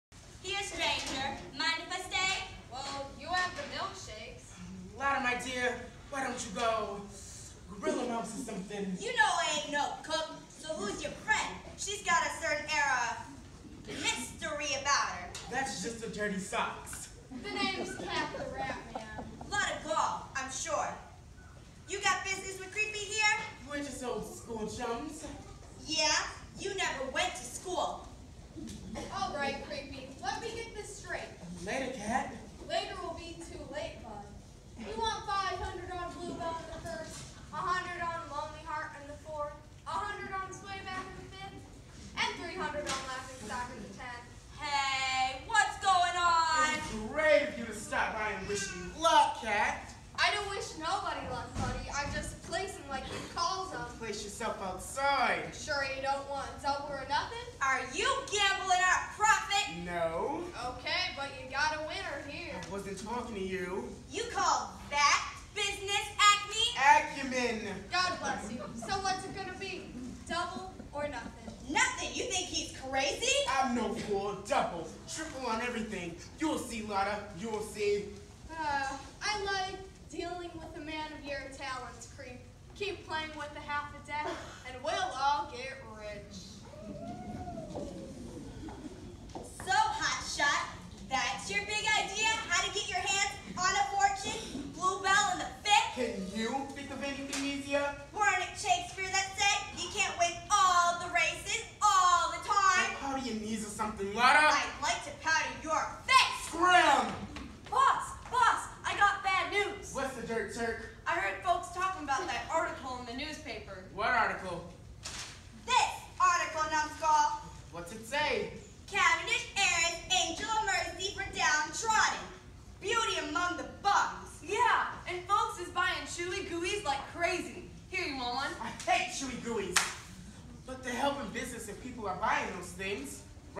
Million Dollar Baby - McBride Middle School #2
Scene with Lotta Gall (Taylor), Creepy and Kat the Rat